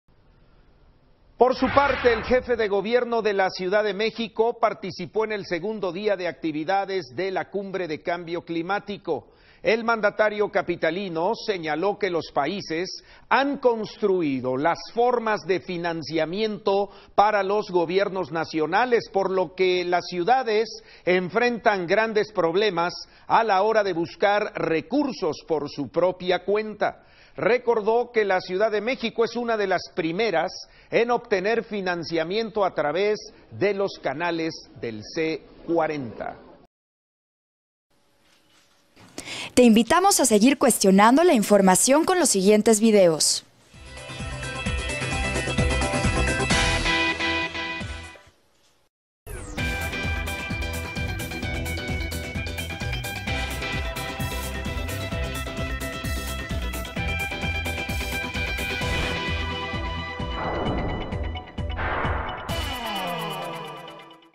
Mancera participa en la C40
Miguel Ángel Mancera, Jefe de Gobierno de la Ciudad de México, participó en la C40 y aseguró que las ciudades enfrentan dificultades para obtener financiamiento.

02 de diciembre 2016